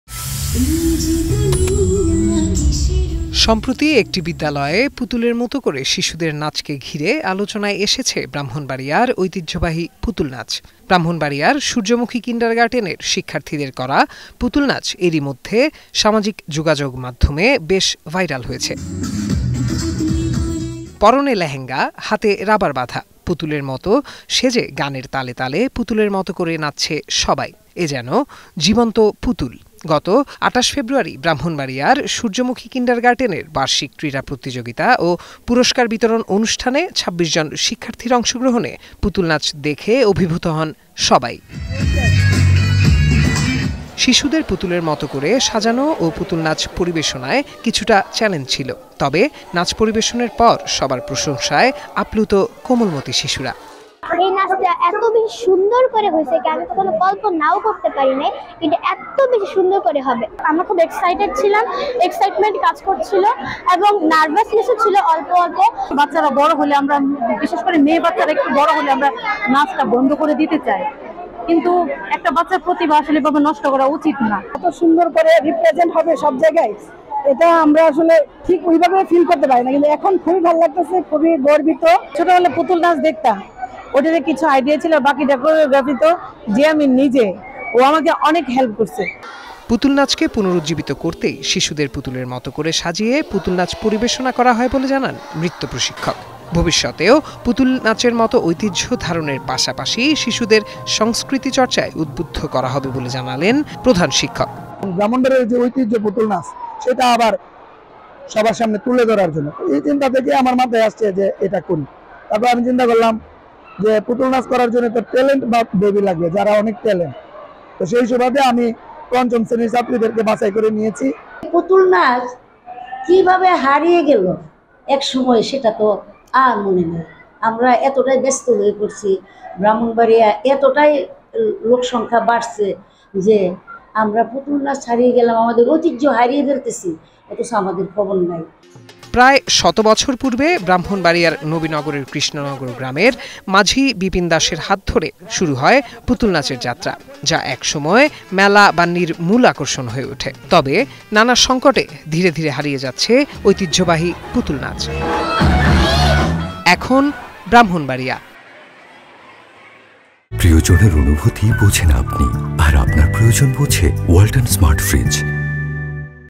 পুতুল সাজে শিশুরা নাচলো ব্রাহ্মণবাড়িয়ার ঐতিহ্যবাহী পুতুল নাচ | Brahmanbaria Putul | Ekhon TV
Our Office Address:
EKHON TV
City Park Lane
19, Hatkhola Road, Wari
Dhaka-1203
Bangladesh